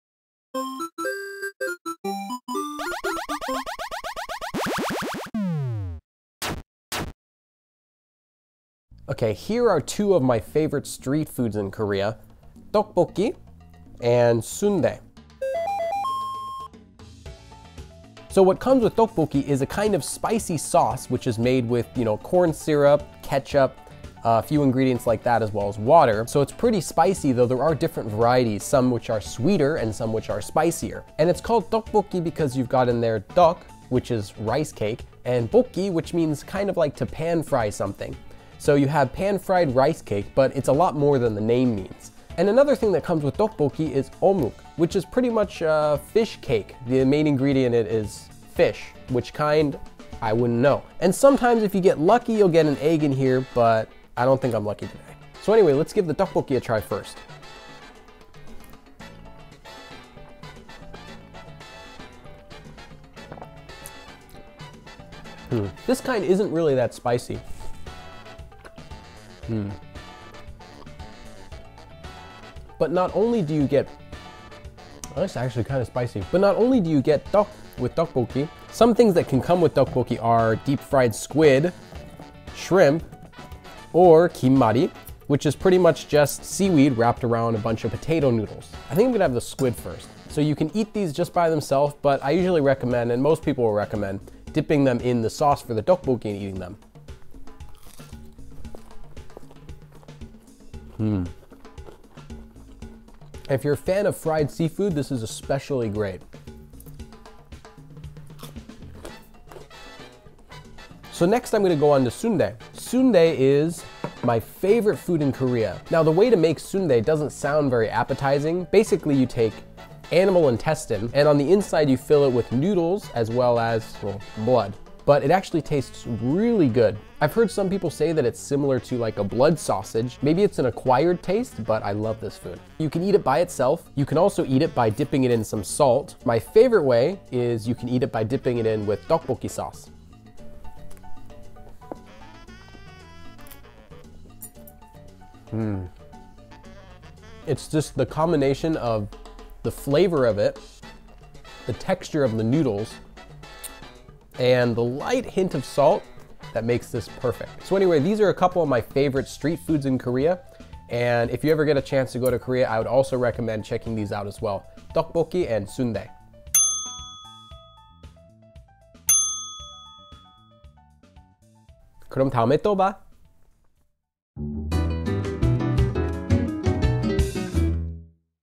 Spicy Korean Tteokbokki (떡볶이) and Sundae (순대)
Find out why Tteokbokki (떡볶이) and Sundae (순대) are two of my favorite Korean foods! This is not the kind of Sundae you might be thinking of (there's no ice cream).